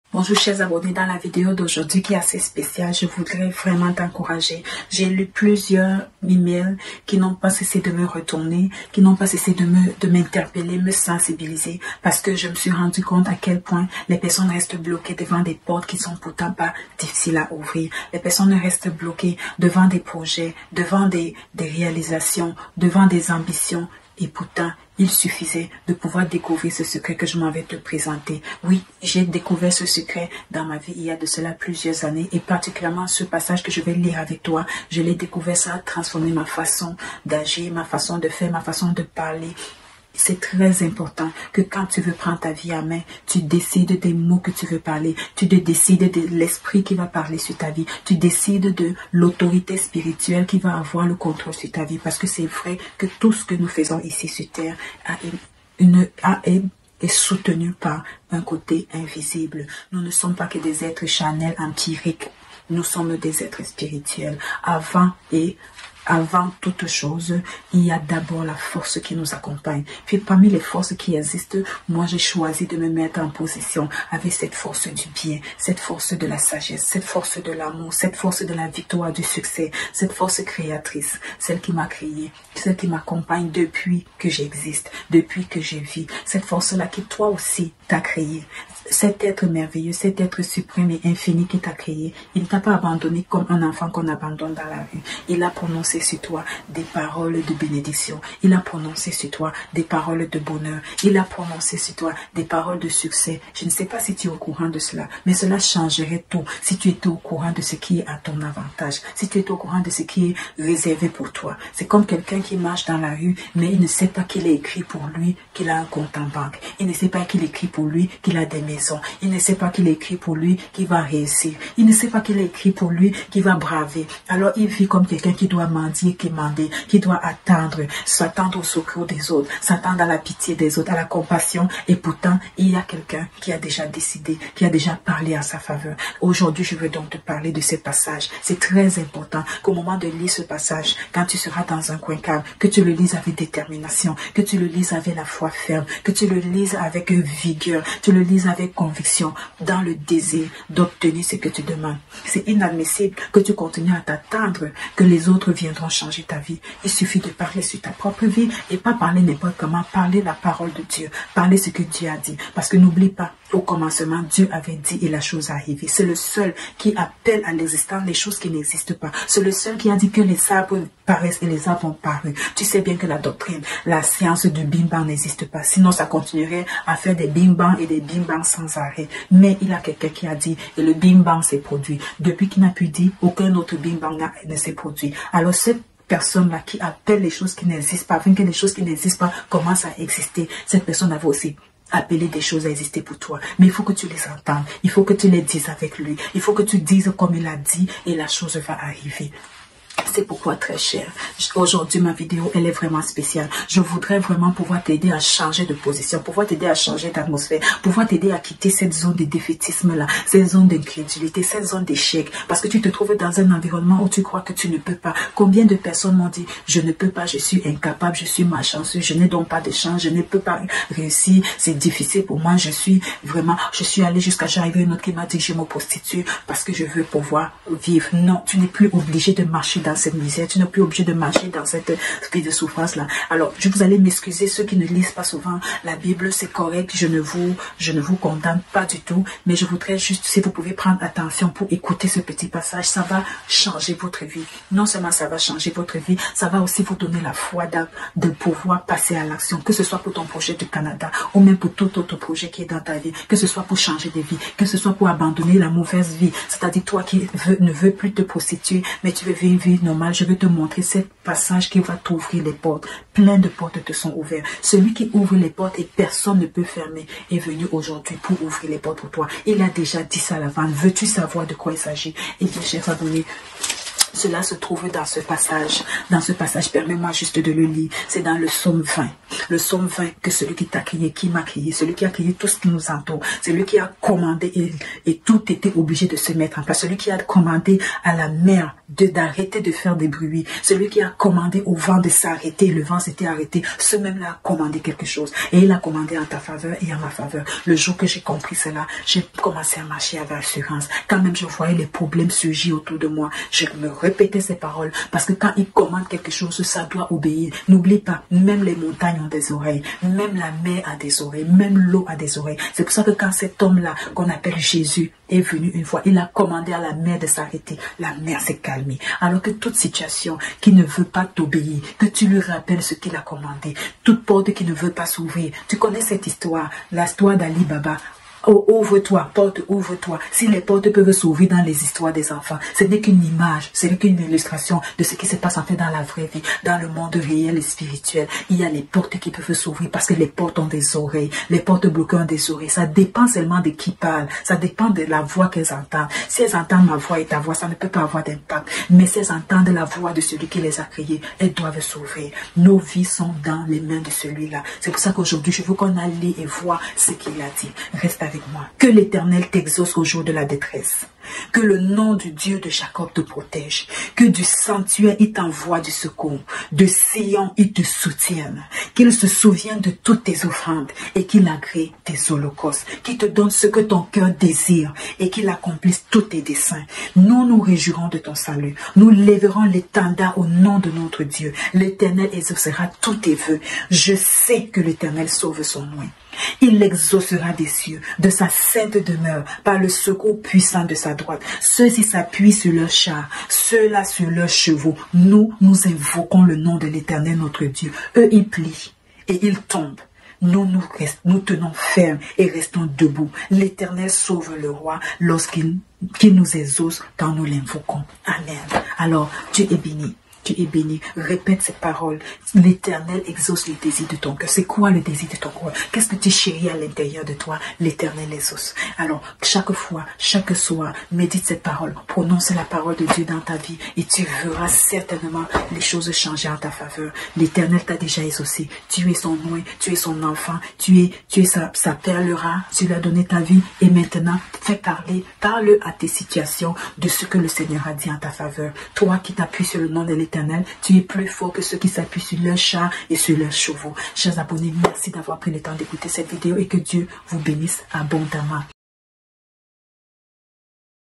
Augmente ta foi en tes possibilités! Épanouissement global
Plusieurs m'ont écrit, plusieurs ont pleuré sur les obstacles qui les empêchent de réaliser leur rêve canadien. Très souvent, le manque de support, de soutien financier, de. Savoir-faire était indexé.
Aujourd'hui, je suis en mission pour t'aider à croire, croire, croire les bonnes paroles qui ont d'avance été dites sur toi!
Crois et passe à l'action, un petit pas à la fois.

Bon visionnement!